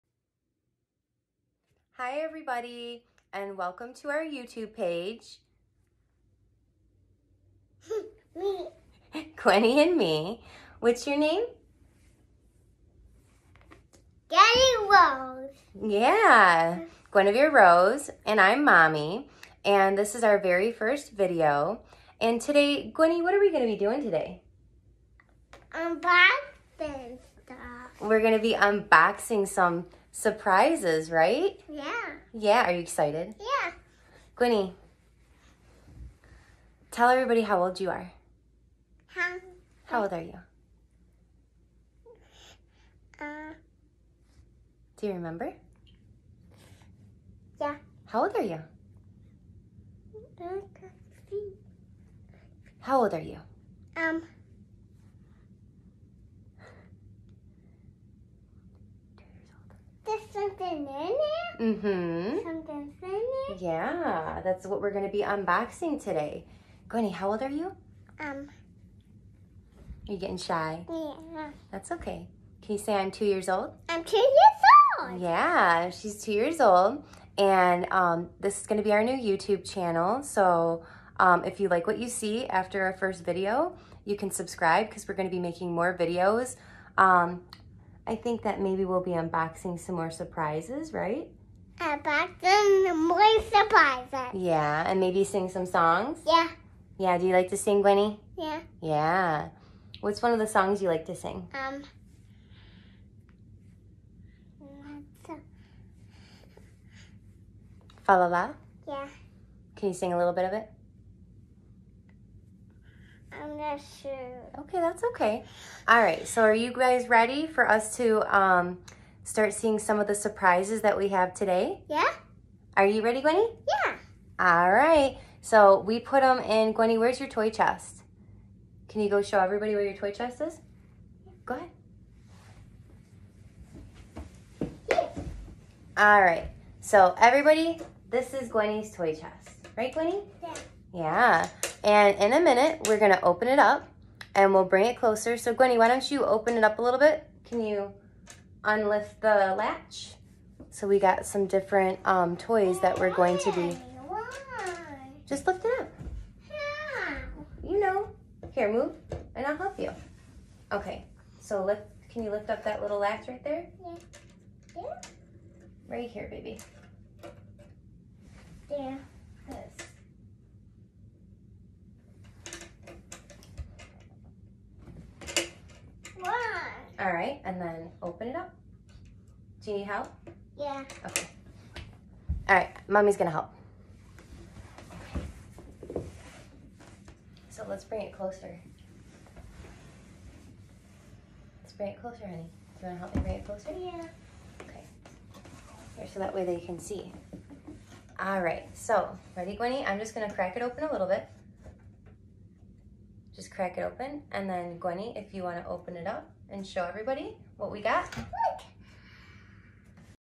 Toy Chest Full of toy Surprises Ready to be Unboxed, Welcome, Surprise Toys
Watch Gwenny open her toy chest full of toy surprises ready to be unboxed and revealed. See what toy surprises are inside of Gwenny’s toy chest. Watch to see what is inside each toy surprise as Gwenny unboxes her surprise toys.